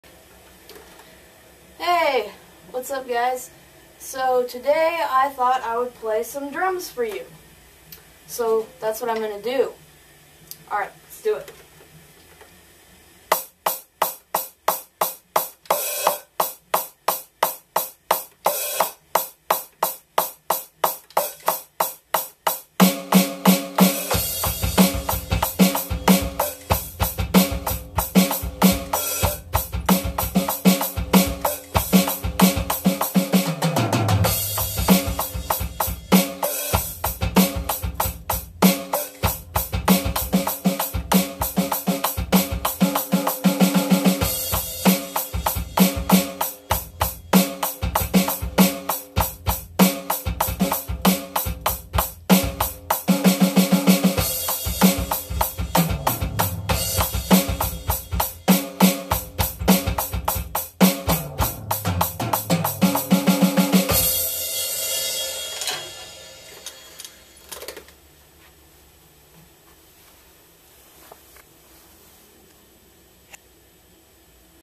Drumming In My Studio
Not only do I make Vlogs, sports, and travel videos, but I also make music videos. I may play more music and different instruments in upcoming videos.
I started playing the drums a few years ago, but I stopped after a while. I started drumming again last year, and I've been learning more beats and tricks ever since then.